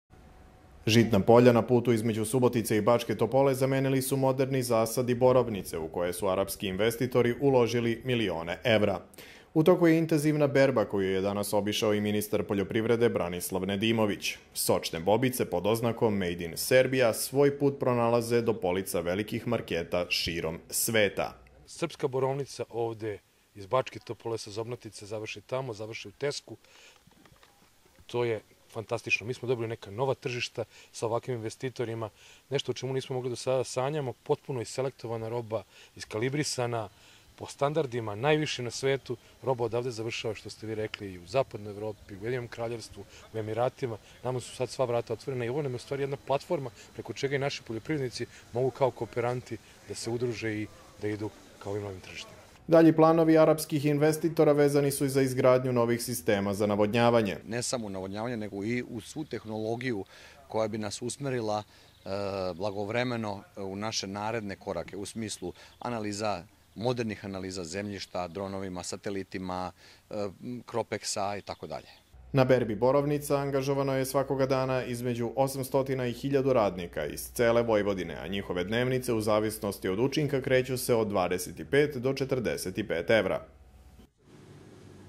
Arapi u Srbiji proizvode borovnice koje se izvoze širom Evrope
Veliki inostrani investitiori u oblasti poljoprivrede prilika su za domaće proizvođače za povezivanje i plasman na njihova tržišta, ocenio je ministar poljoprivrede Branislav Nedimović. On je danas obišao plantažu borovnica u Zobnatici kod Bačke Topole, koju arapski investitiori izvoze širom Evrope. Arapi u Srbiji proizvode i veliko količine krompira i voljni su da se povežu sa drugim domaćim proizvođačima radi zajedničkog plasmana.